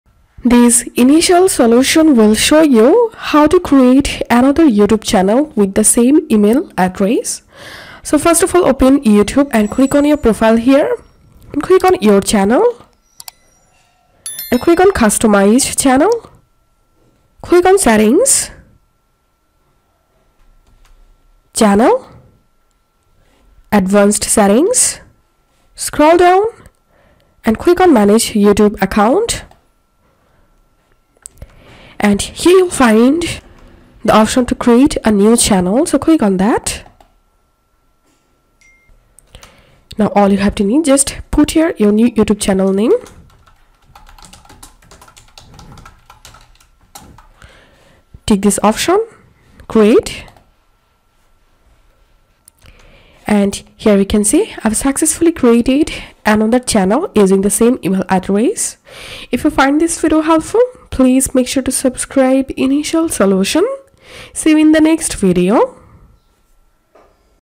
How to create another YouTube channel with the same email 2024 | Initial Solution
Step 1: Then go to the Youtube Channel switcher and click ‘Create a new channel.’ -you are going to see a screen.
Step 2: Here, you need to think of your brand account name and type it into the large white box before clicking ‘create.’ It may be a good idea to click on the link and remind yourself of them.
step 3: Once you have clicked ‘create,’ you will be taken to the next screen. This is the page for your new channel.